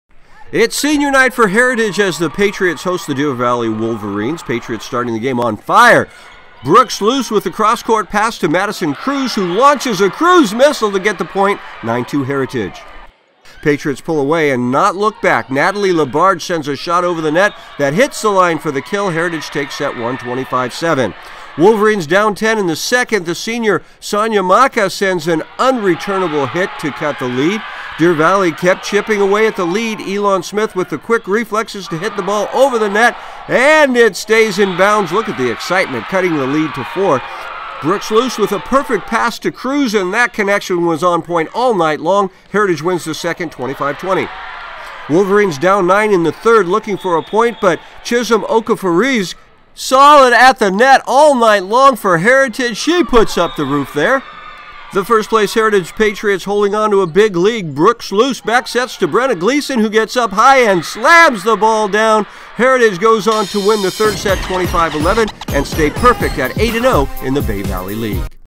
Deer Valley at Heritage Girls Volleyball 10.13.22
IT’S SENIOR NIGHT AT HERITAGE AS THE PATRIOTS HOST THE DEER VALLEY WOLVERINE.

PATRIOTS STARTED THE GAME ON FIRE, BROOKE SLOUS WITH A CROSS COURT PASS TO MADDISON KRUSE WHO LAUNCHES A KRUSE MISSILE TO GET THE POINT, 9-2 HERITAGE.

PATRIOTS WOULD PULL AWAY AND NOT LOOK BACK, NATALIE LABARGE SENDS A SHOT OVER THE NET THAT HITS THE LINE FOR THE KILL, HERITAGE TAKES SET ONE, 25-7.

WOLVERINES DOWN 10 IN THE SECOND SET, THE SENIOR SANIYA MAKA SEND AN UNRETURNABLE HIT TO CUT THE LEAD.

DEER VALLEY KEPT CHIPPING AWAY AT THE LEAD, ELON SMITH WITH THE QUICK REFLEXES TO HIT THE BALL OVER THE NET AAAAAAAND IT STAYS IN BOUNDS. LOOK AT THE EXCITEMENT. CUTS THE LEAD TO 4.

BUT THE PATRIOTS STOOD THEIR GROUND, BROOKE SLOUS WITH A PERFECT PASS TO KRUSE AND THIS CONNECTION WAS ON POINT THE WHOLE NIGHT. HERITAGE WINS THE SECOND SET, 25-20.

WOLVERINES DOWN 9 IN THE THIRD SET, LOOKING FOR A POINT BUT CHISOM OKAFORIZE GIVES DEER VALLEY A BLOCK.

HERITAGE STILL HOLDING ONTO A BIG LEAD, BROOKE SLOUS BACK SETS TO BRENNA GLEASON WHO GETS UP HIGH AND SLAMS THE BALL DOWN. HERITAGE GOES ON TO WIN THE THIRD SET 25-11 AND STAY A PERFECT 8-0 IN THE BAY VALLEY LEAGUE.